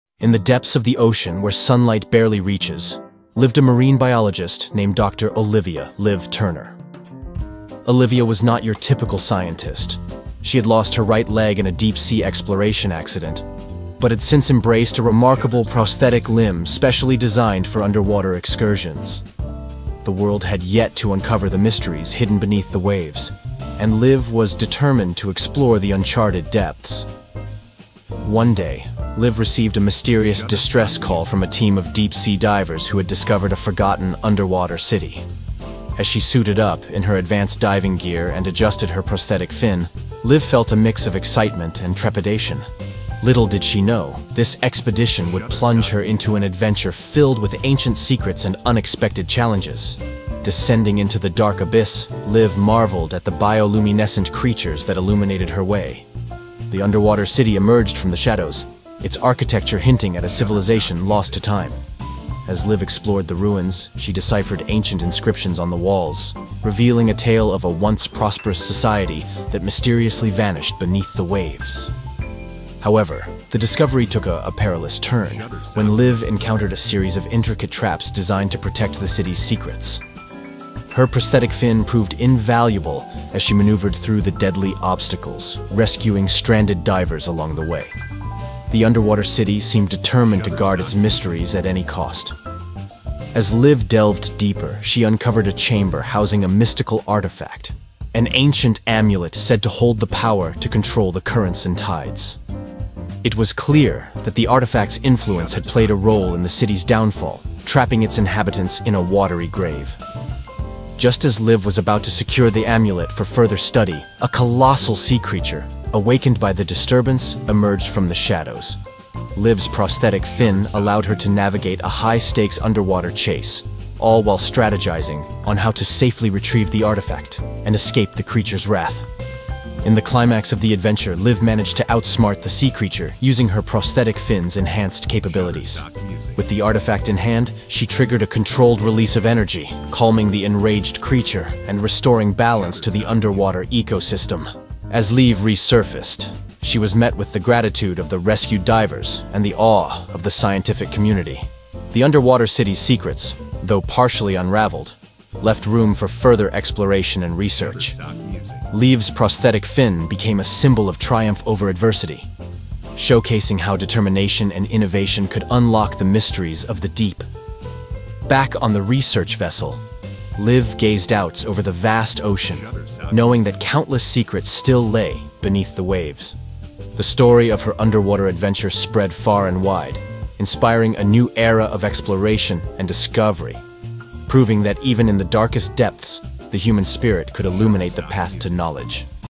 Submerged Secret/Red's facts of life
Dr. Olivia "Liv" Turner, a marine biologist with a prosthetic fin, embarks on an expedition after receiving a distress call about a forgotten underwater city. As she explores the ancient ruins, Liv unravels the mystery of a vanished civilization and encounters deadly traps protecting the city's secrets. She discovers a mystical artifact, the cause of the city's downfall, and faces a colossal sea creature guarding it. Using her prosthetic fin, Liv outsmarts the creature, retrieves the artifact, and restores balance to the underwater ecosystem. Her triumph becomes a symbol of determination and innovation, inspiring a new era of exploration. The story ends with Liv contemplating the vast ocean and the countless secrets that still await discovery./Submerged Secret/Red's facts of life